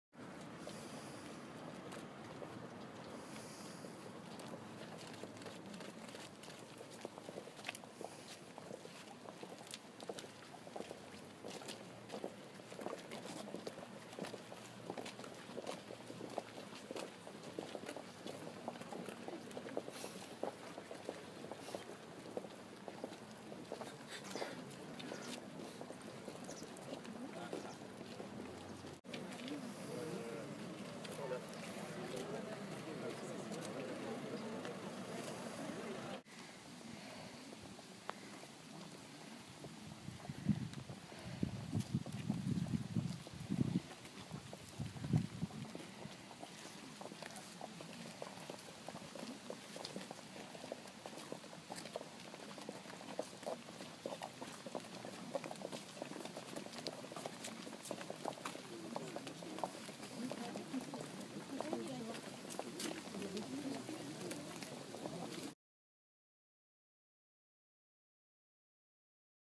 Lemainelibre.fr : Marche blanche à Tuffé
Le 18 août dernier, Mickaël Lambert, 39 ans, père d'un garçon, signaleur bénévole, était renversé par une voiture en marge des festivités des Tufféeries. Il est décédé deux semaines après, le 31 août.

120 personnes sont rassemblées ce vendredi à Tuffé, pour la marche blanche organisée en son hommage et qui vient de s'élancer à 17h20 de la mairie jusqu'au lieu de l'accident, situé à 1,7 km.